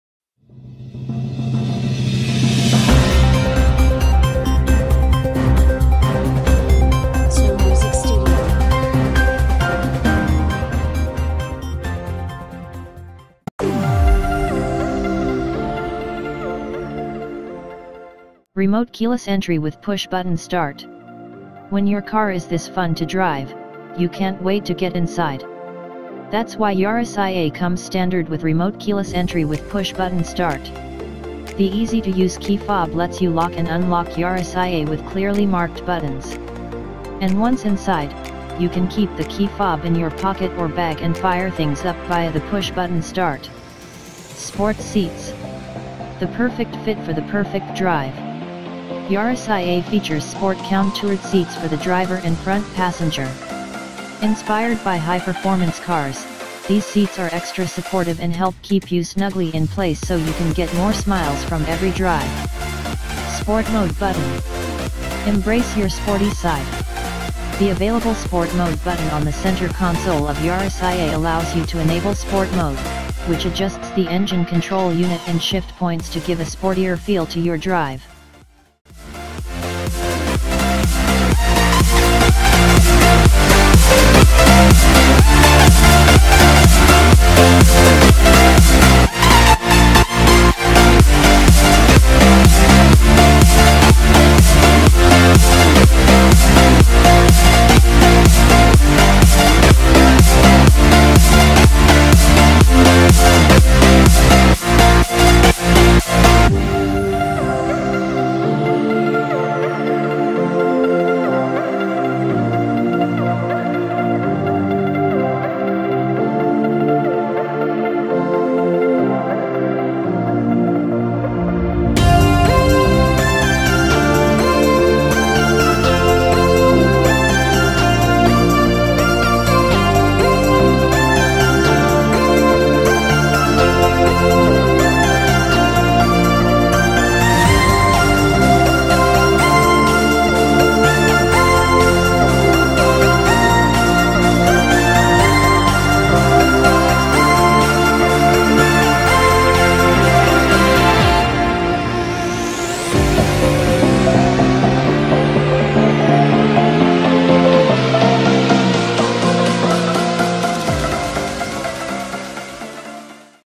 2018 Toyota Yaris iA. Style, tech and a whole lot more. Standard
2018 Toyota Yaris iA. Style, tech and a whole lot more. Standard

Remote keyless entry with Push Button Start
When your car is this fun to drive, you can't wait to get inside. That's why Yaris iA comes standard with remote keyless entry with Push Button Start. The easy-to-use key fob lets you lock and unlock Yaris iA with clearly marked buttons. And once inside, you can keep the key fob in your pocket or bag and fire things up via the Push Button Start.

Sports seats
The perfect fit for the perfect drive. Yaris iA features sport-countoured seats for the driver and front passenger. Inspired by high-performance cars, these seats are extra supportive and help keep you snugly in place so you can get more smiles from every drive.

Credit Sound : NCS